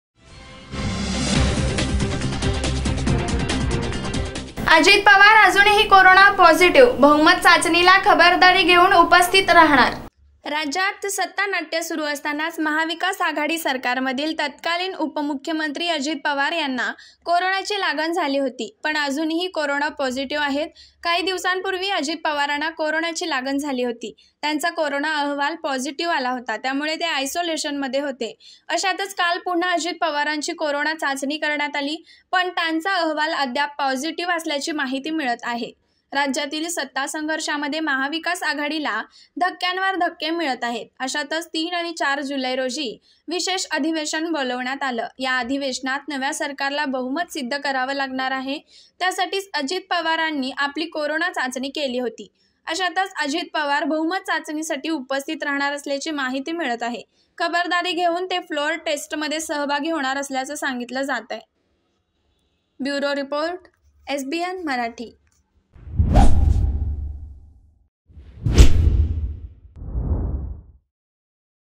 AJIT PAWAR | अजित पवार अजूनही कोरोना पॉझिटिव्ह; बहुमत चाचणीला खबरदारी घेऊन उपस्थित राहणार
अजित पवार अजूनही कोरोना पॉझिटिव्ह; बहुमत चाचणीला खबरदारी घेऊन उपस्थित राहणार